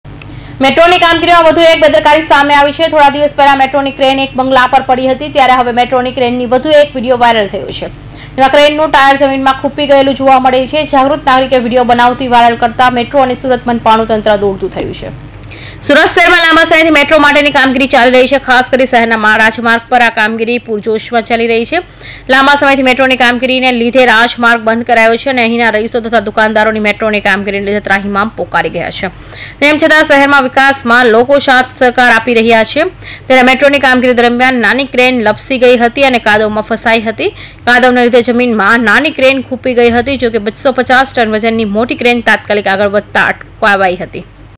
સુરતમાં મેટ્રોની કામગીરીમાં વધુ એક બેદરકારી,
રાજમાર્ગ પર ક્રેઈનનું ટાયર જમીનમાં ખૂંપી ગયું